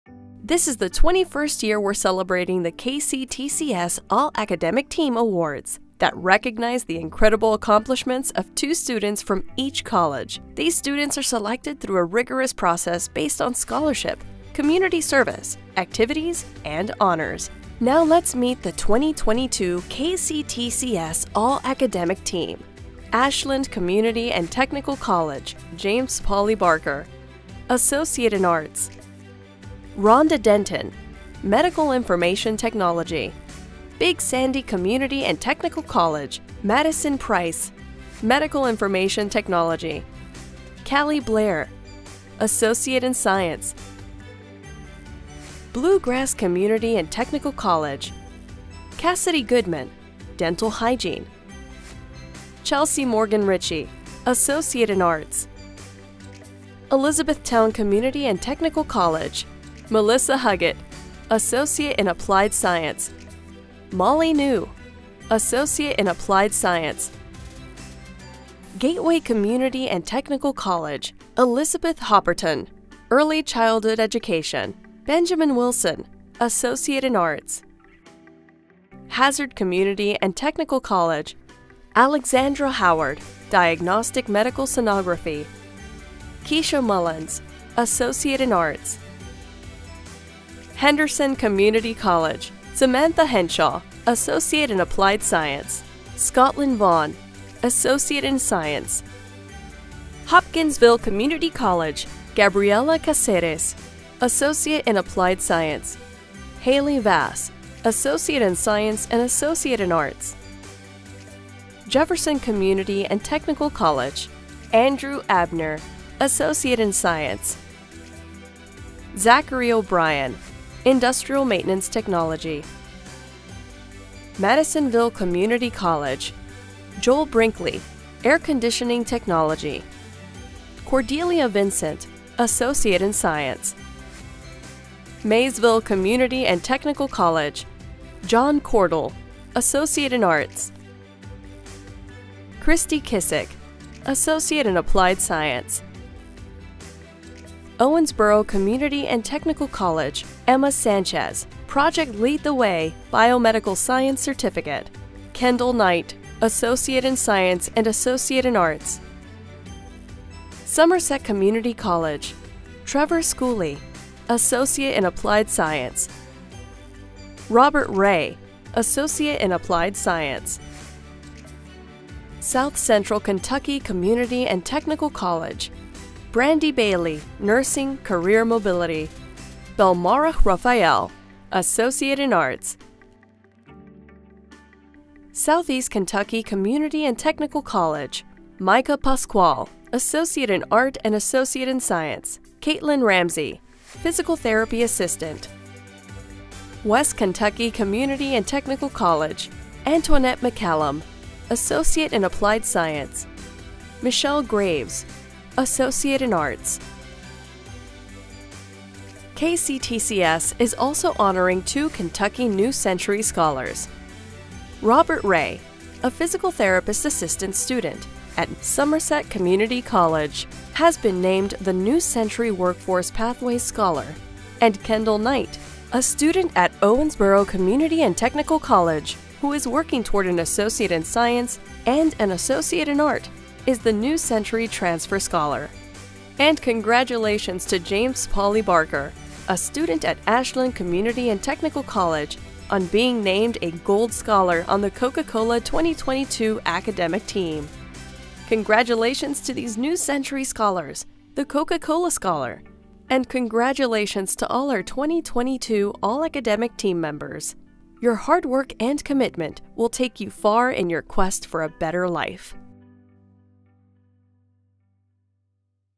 The 2022 KCTCS All-Academic Team
Introducing the 2022 KCTCS All-Academic Team from each of the 16 Colleges.